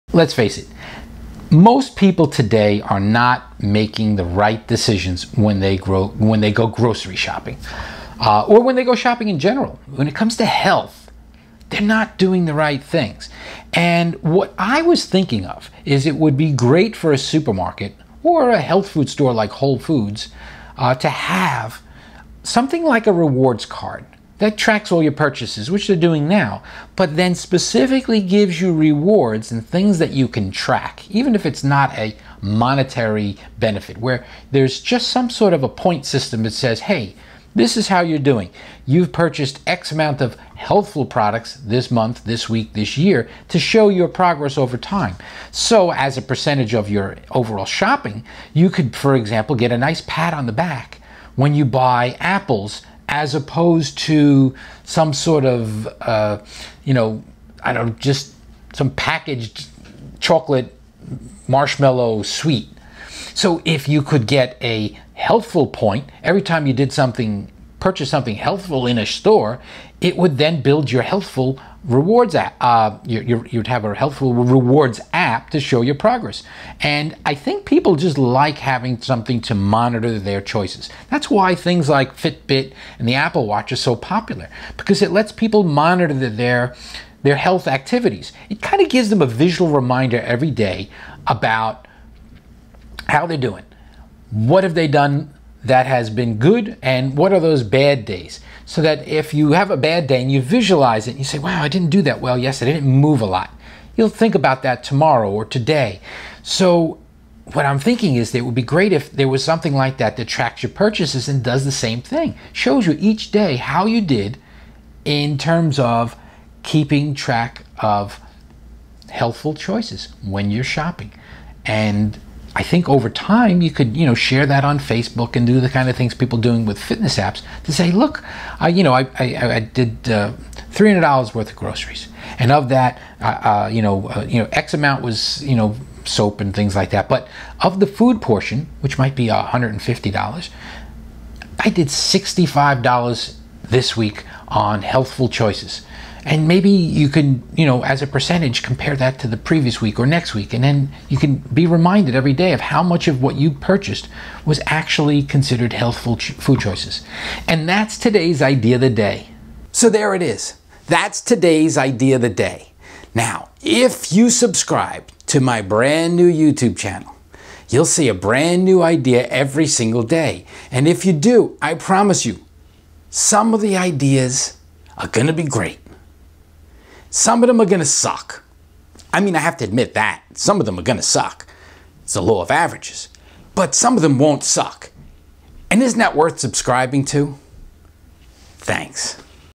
A Healthful Choices Rewards Program. Idea of the day #118
Whenever people track their health choices, they tend to make better decisions.

One of the best things about devices like Fitbit and the Apple Watch is that it motivates people to move around more often, if nothing else, because they now have a simple way to track their progress. Simply stated, when people track something, they tend to pay attention to the little decisions they make each day.

While devices like that are great for monitoring your activity, I think it would be great to have a way to track other health choices that people make, such as their grocery shopping.

With a Healthful Choices Rewards Card, you would earn points every time you made a healthful purchase. For example, you would earn more points for buying Apples than you would for buying Twinkies. Taken to the next level, if you could use the rewards card in more than one business, you would earn points in restaurants, and even when you work out at the gym.

This was featured as the July 7, 2016 Idea Of The Day.